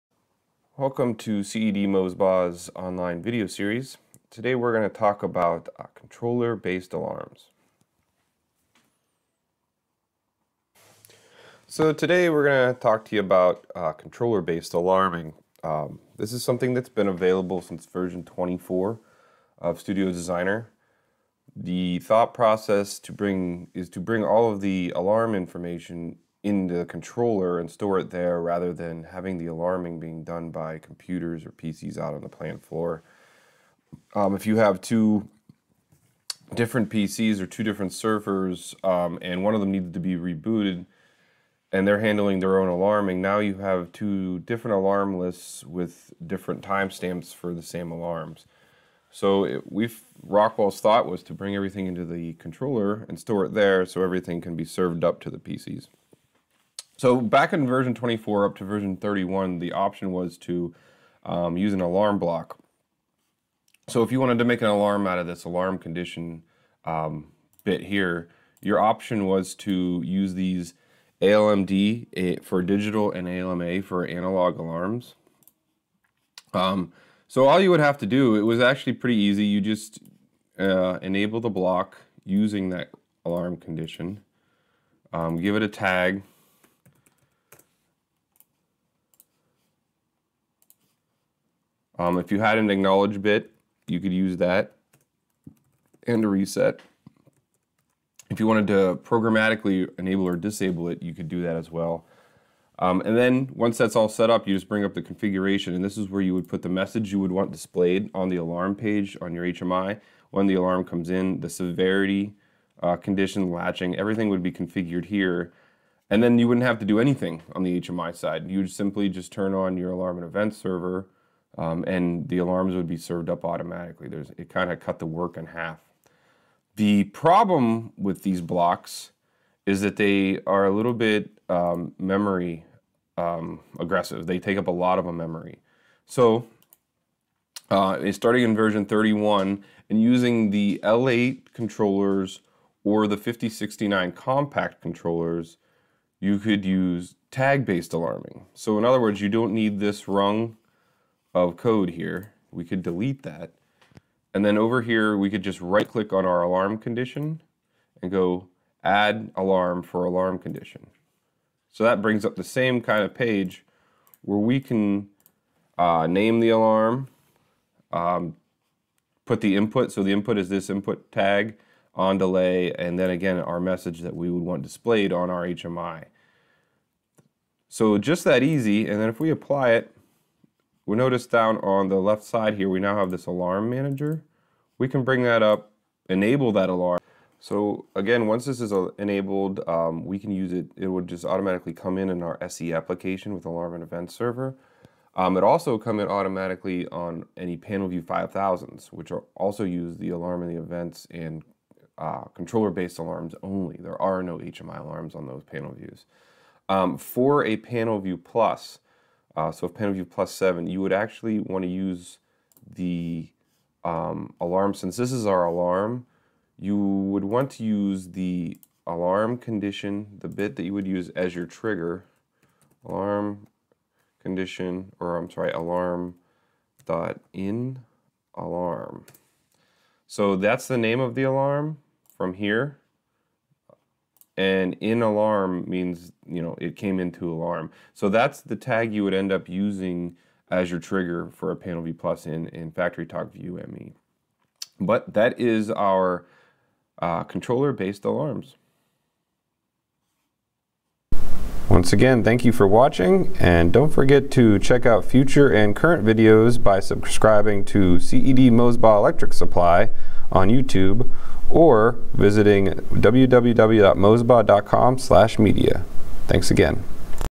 Controller Based Alarms
Using Controller based alarms in Studio 5000 Logix Designer